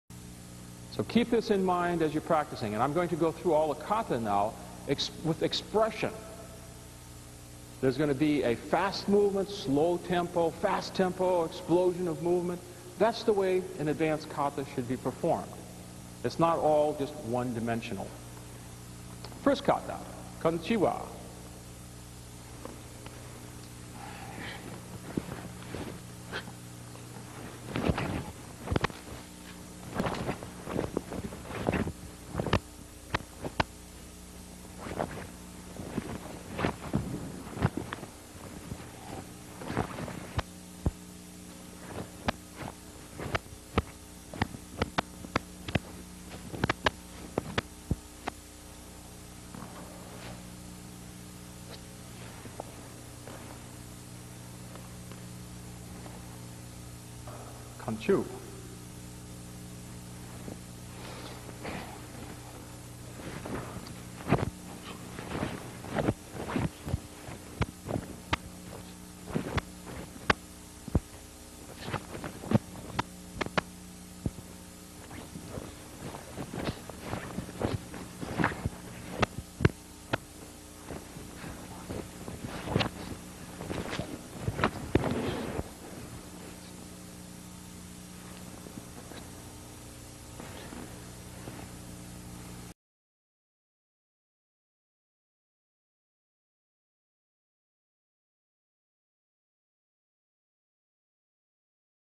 Sensei George Mattson - Uechi Ryu Kanshiwa & Kanshu Kata
Sensei Mattson, 10th Dan Uechi Ryu performs Kanshiwa and Kanshu Kata.
Video is from Approximately late 1990s from Sensei's VidMag Instructional series.

Videos could be anything.  High School Sports, Youth sports, Drone Videos, Martial Arts, POLO, Fireworks, and who knows what else.